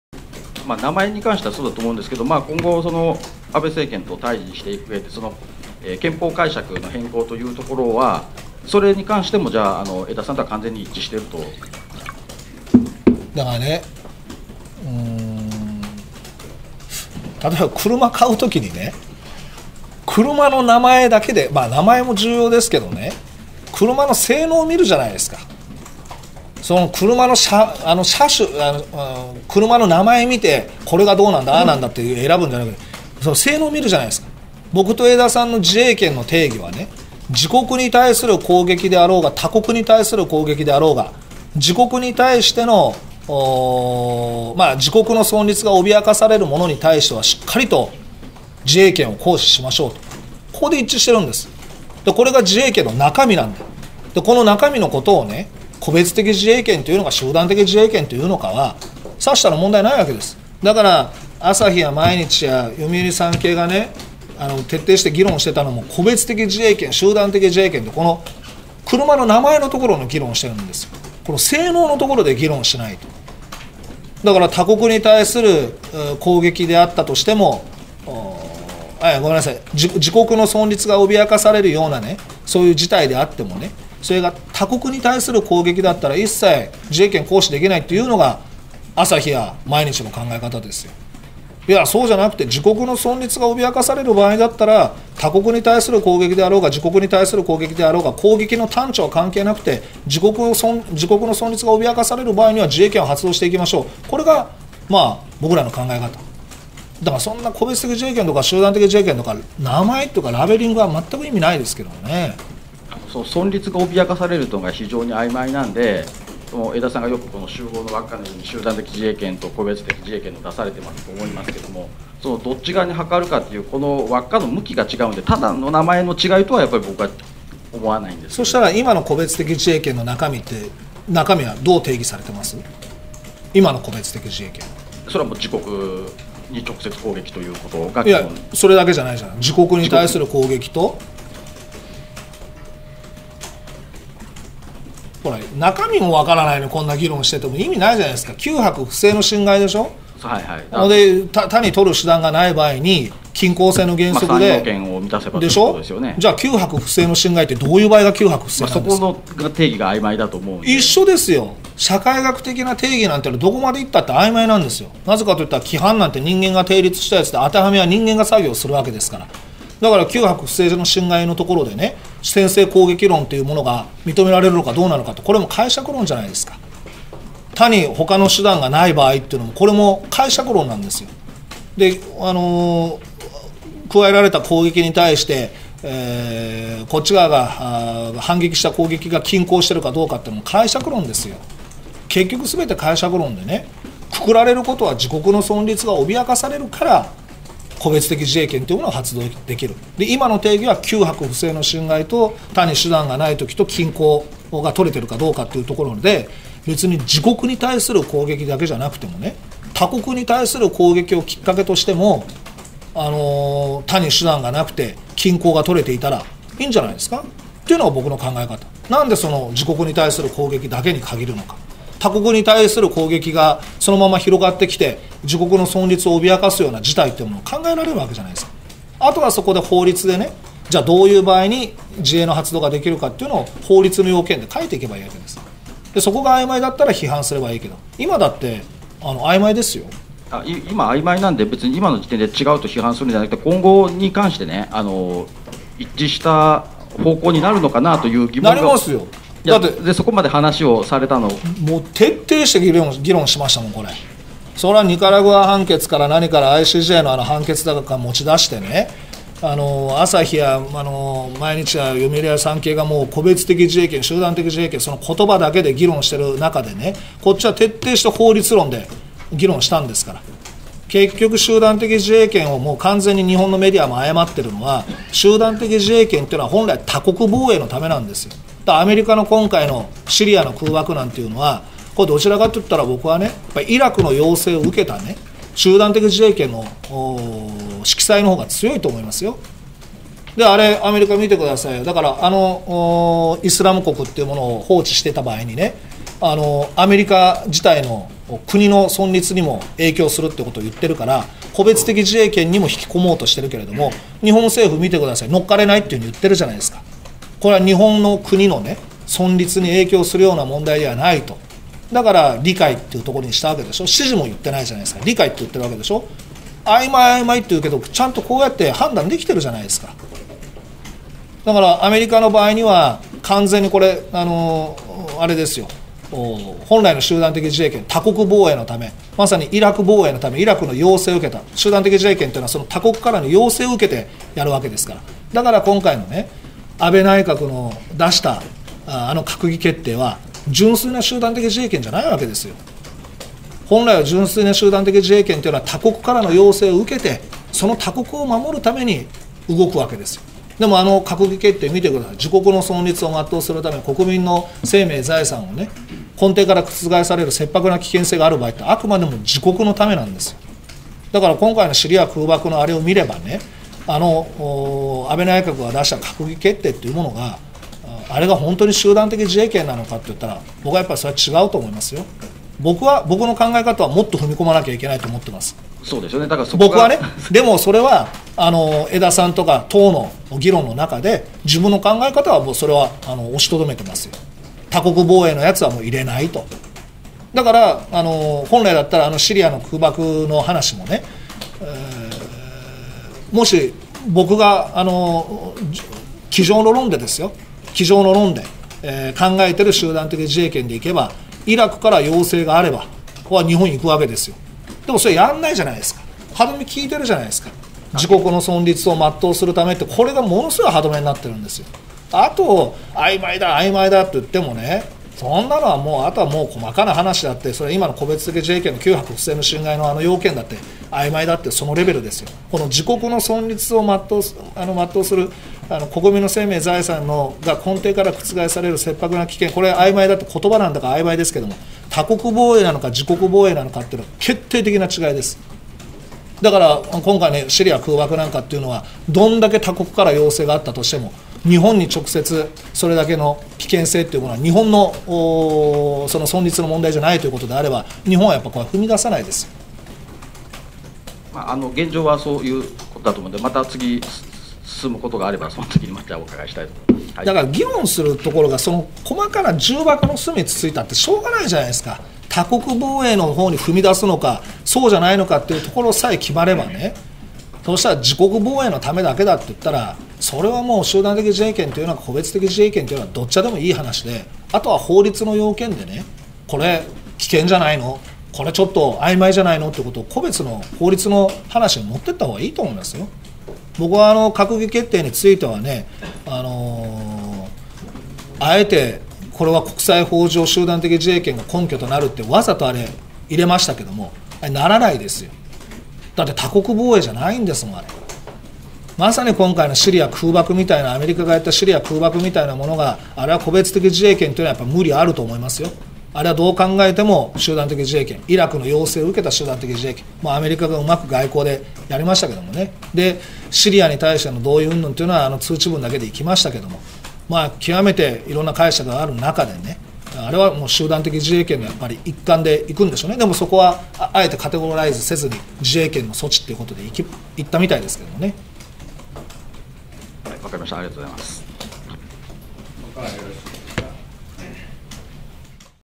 橋下徹の逆質問にMBSの奥田記者が思考停止した瞬間
2分15秒あたり
集団的自衛権と個別的自衛権の違い
橋下大学　橋下塾　江田憲司　維新の党